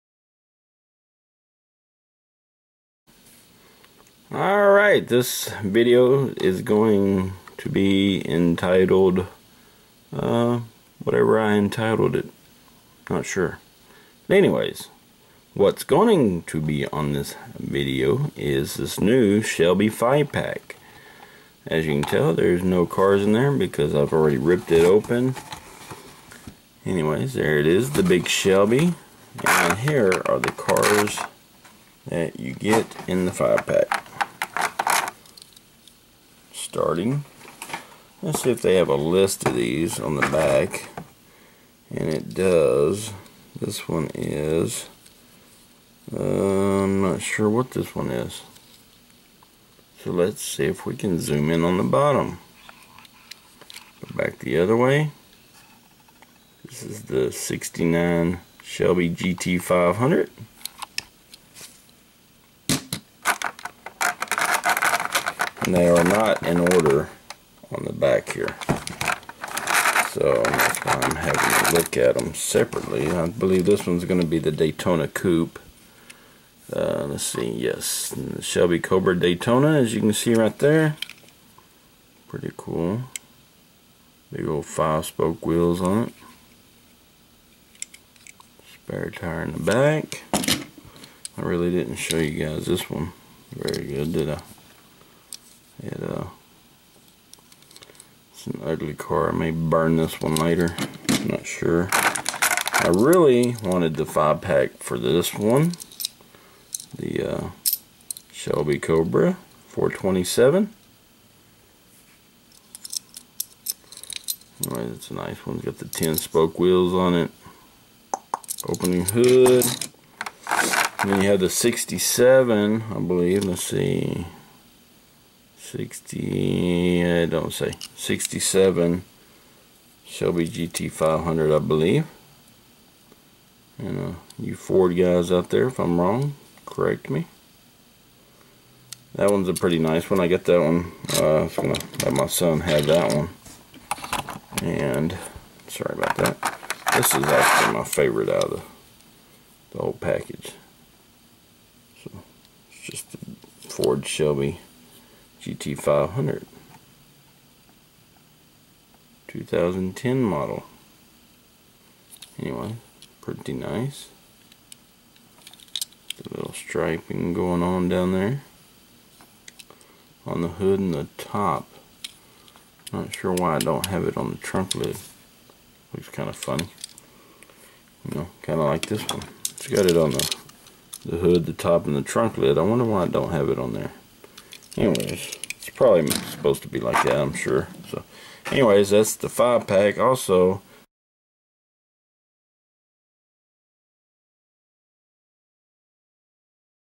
Hot Wheels : Ford Shelby Mustang Gift Pack
My wife picked this up for me as an extra Birthday bonus. This did come from Walmart but I have seen it since at other retailers.